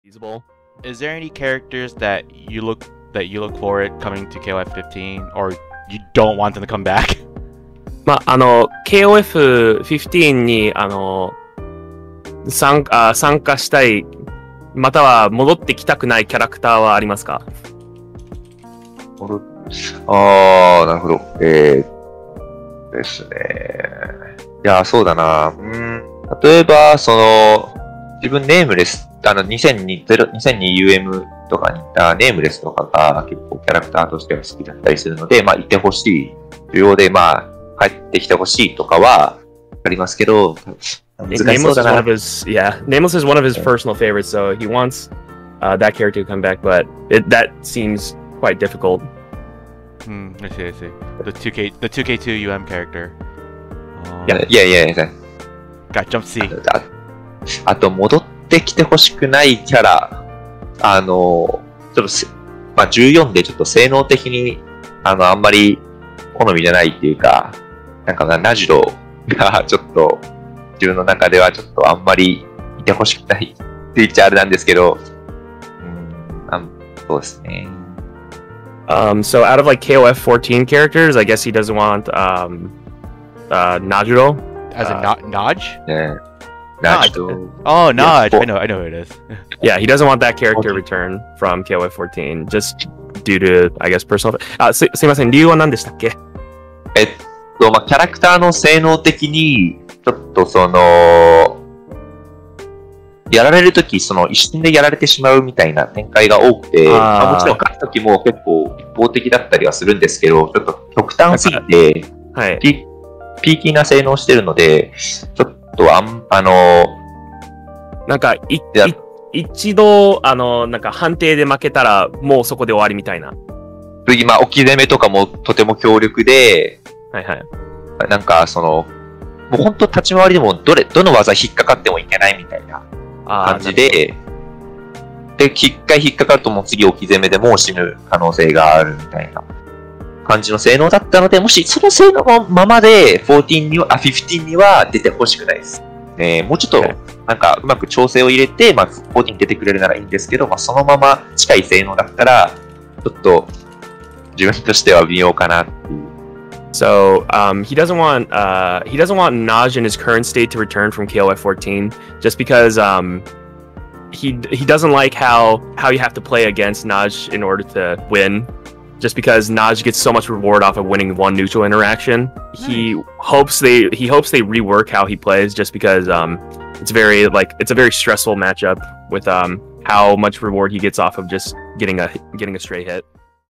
Why Laggia doesn't want this character to return in KOFXV
Laggia explains why he doesn't want this character to return in KOFXV